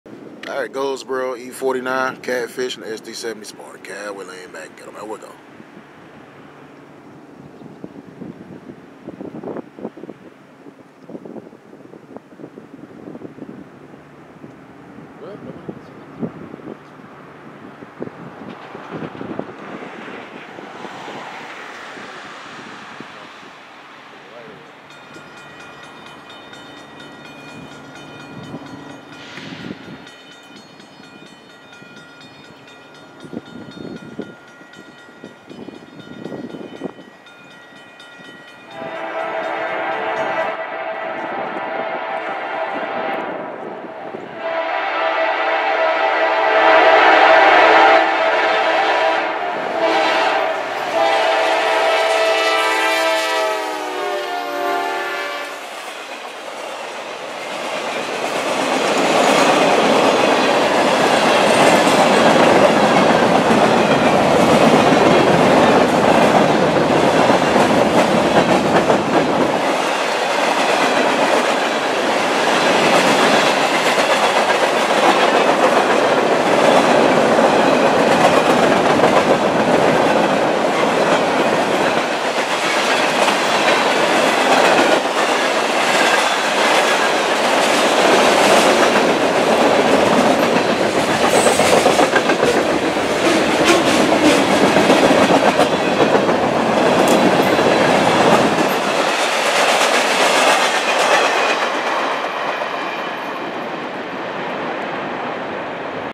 NS E49 With an C40-9W/SD70 Standard Cab Track Speed Rolling good K5LA Horn
Eastbound By Goldsboro NC Headed to Choco NC 1/12/16